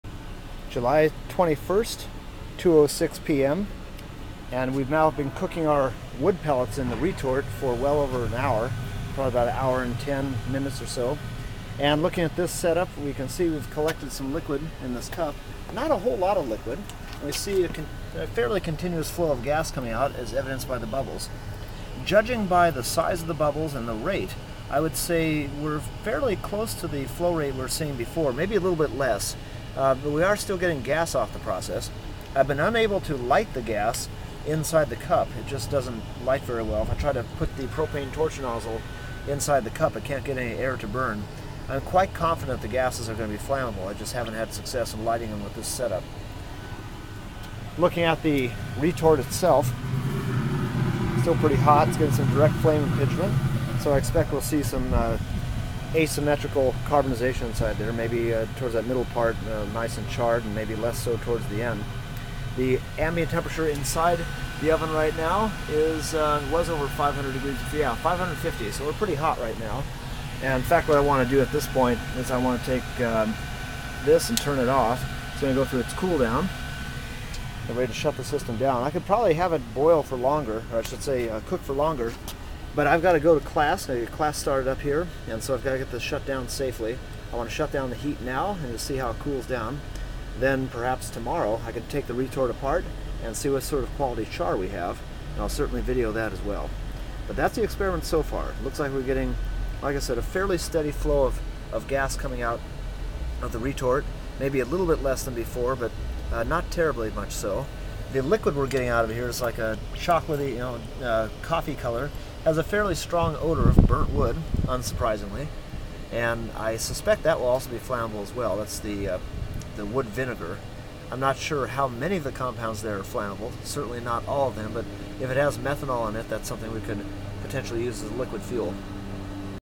Biomass pyrolysis experiment -- retort fired by grill (phase 5)
This is one in a series of video clips showing a simple qualitative experiment pyrolyzing wood pellets.  Pyrolysis is the "destructive distillation" of organic matter by the application of heat, usually in a low-oxygen environment.  In this, case, I am testing the practicality of pyrolyzing wood pellets in a retort made of black iron pipe, capturing the gases and liquids that escape for further analysis.

Our long-range plan is to build some sort of biomass pyrolysis/gasification unit as a platform for applying industrial instrumentation and control, which is the subject of this program at BTC.